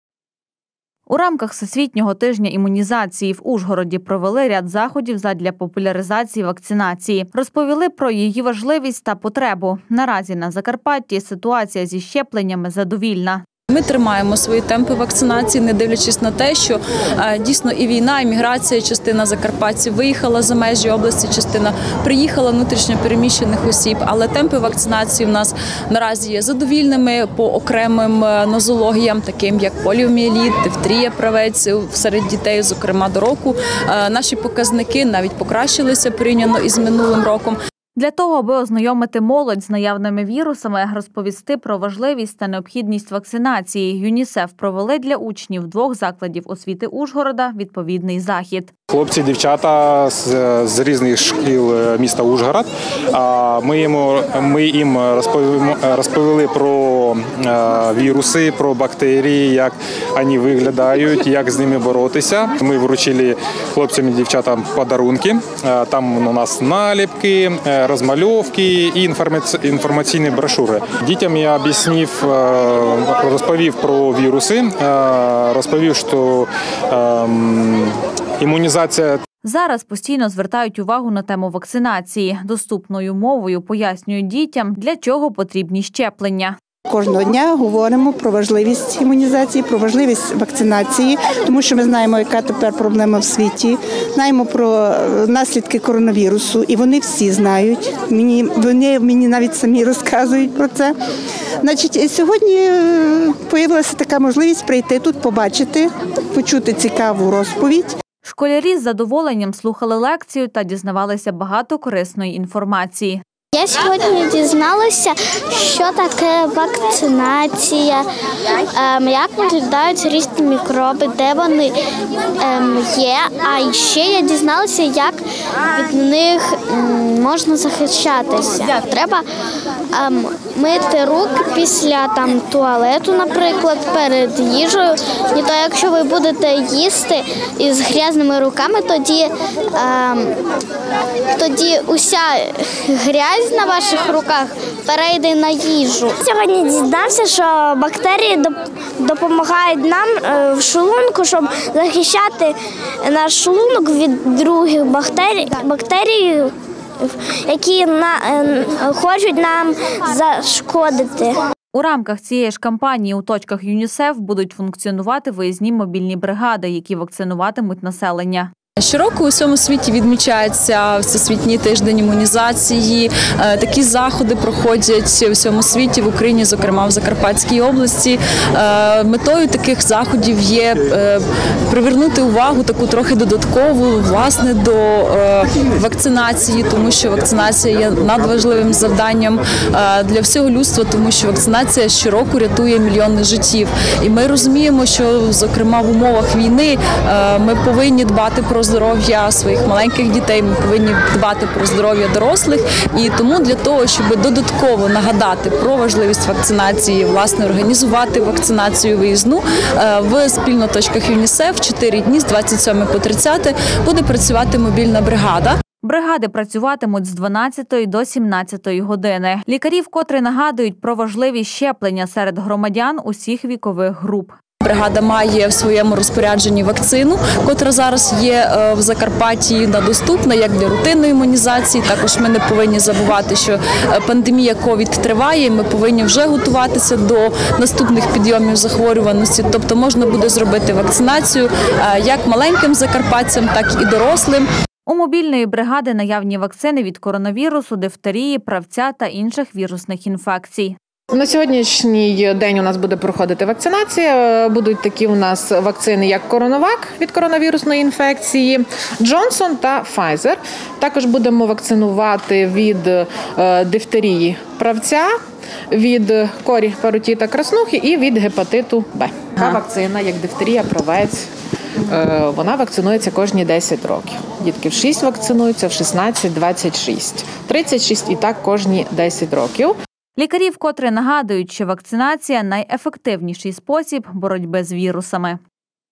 Всесвітній тиждень імунізації
Задля популяризації імунізації та інформування молоді про наявні віруси у Совиному гнізді за підтримки ЮНІСЕФ  провели захід з учнями закладів освіти міста. Відвідали цікаву зустріч і наші журналісти. Більше про те чи знають школярі про небезпечні віруси та як дбати про своє здоров’я. У рамках Всесвітнього тижня імунізації в Ужгороді провели ряд заходів задля популяризації вакцинації. Розповіли про її важливість та потребу. Наразі на Закарпатті ситуація зі щепленнями задовільна.  Для того аби ознайомити молодь з наявними вірусами, розповісти про важливість та необхідність вакцинації ЮНІСЕФ провели для учнів двох закладів освіти Ужгорода відповідний захід. Зараз постійно звертають увагу на  тему вакцинації. Доступною мовою пояснюючи дітям, для чого потрібні щеплення.  Школярі з задоволенням слухали лекцію та дізналися багато корисної інформації.   У рамках цієї ж  кампанії у точках ЮНІСЕФ будуть функціонувати  виїзні мобільні бригади, які вакцинуватимуть населення.  Бригади працюватимуть з 12 до 17 години. Лікарі вкотре нагадують, про важливість щеплення серед громадян усіх вікових груп.  У мобільної бригади наявні вакцини від коронавірусу, дифтерії, правця та інших вірусних інфекцій.  Лікарі вкотре нагадують, що вакцинація – найефективніший спосіб боротьби з вірусами.